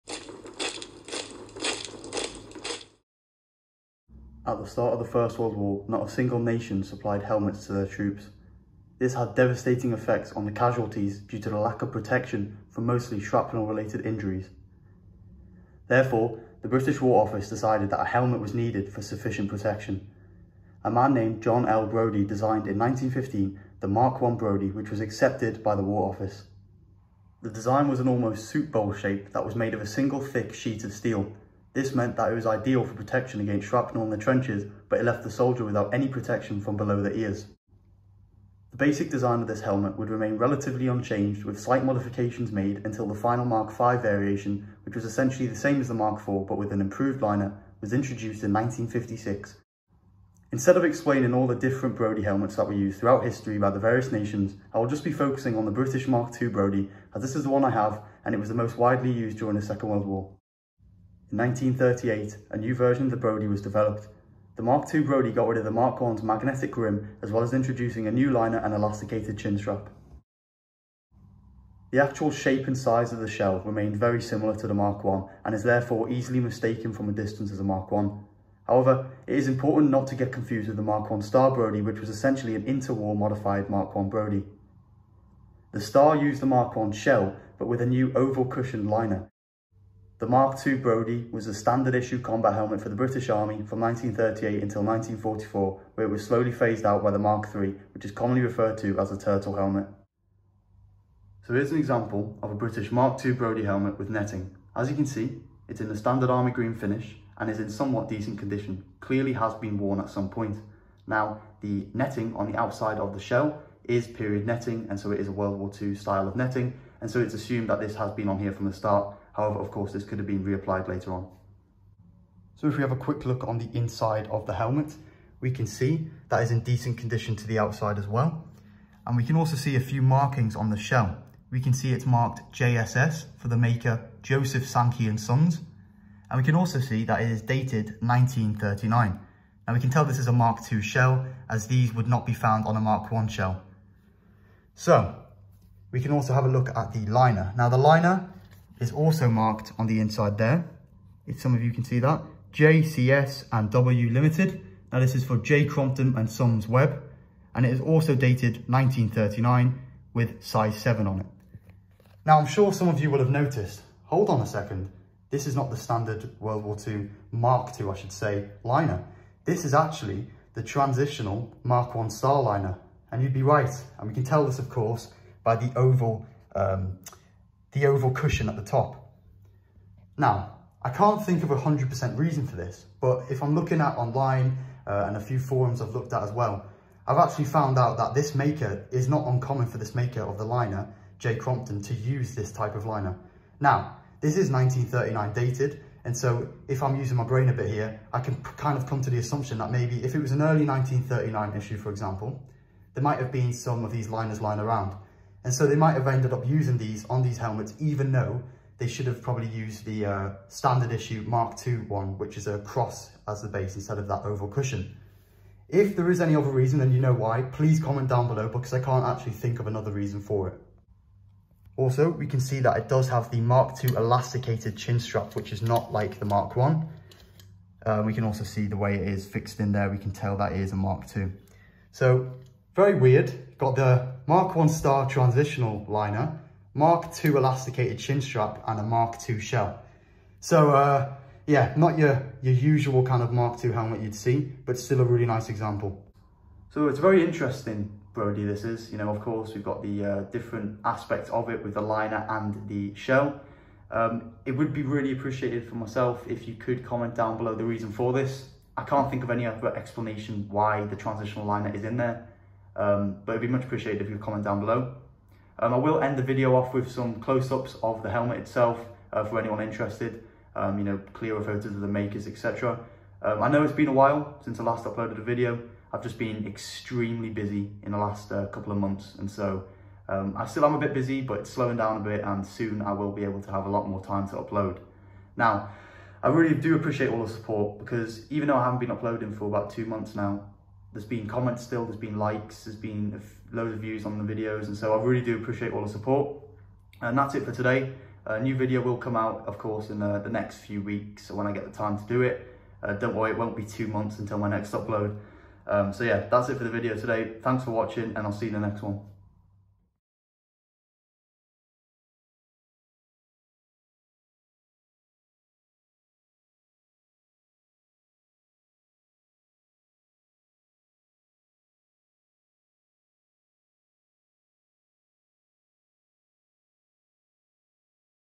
WW2 British Mk2 Brodie Helmet
In this video I will briefly explain the origins of the Brodie helmet as well as going into detail and showing you an example about/of the British Mk2 brodie.

❗️Feel free to drop a comment on the video